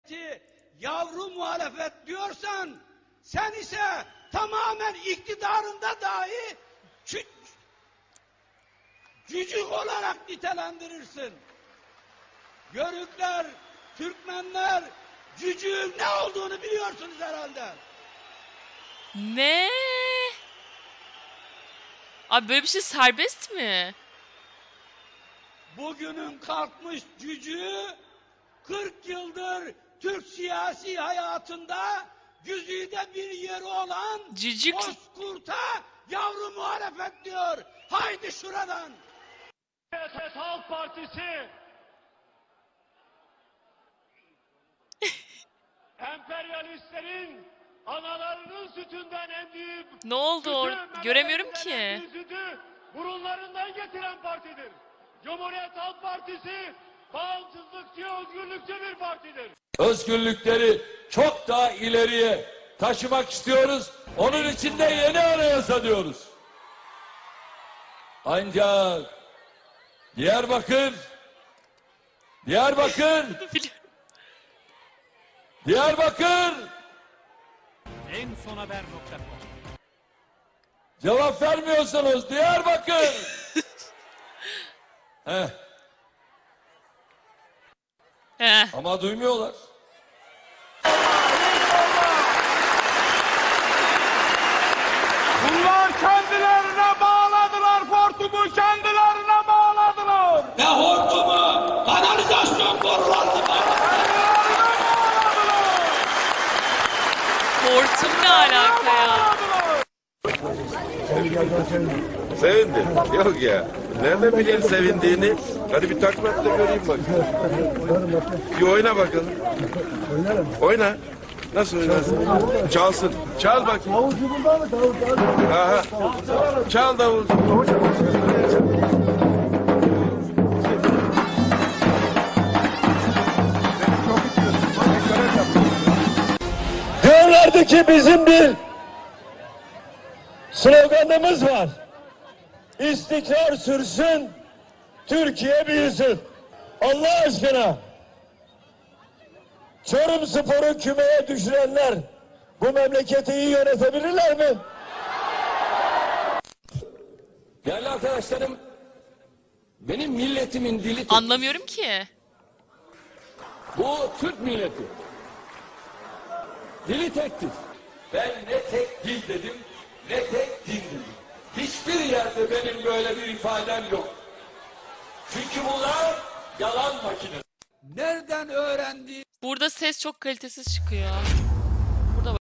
AnnaDeniz - TÜRKİYEDE Kİ SİYASETÇİLERİN KOMİK ANLARINI İZLİYOR (OHAA)
Herkese merhaba arkadaşlar bugün sizlerle birlikte twitchdeki olayları izliyoruz.

Bu arada zil butonuna tıklarsanız her video attığımda haberdar olabilirsiniz!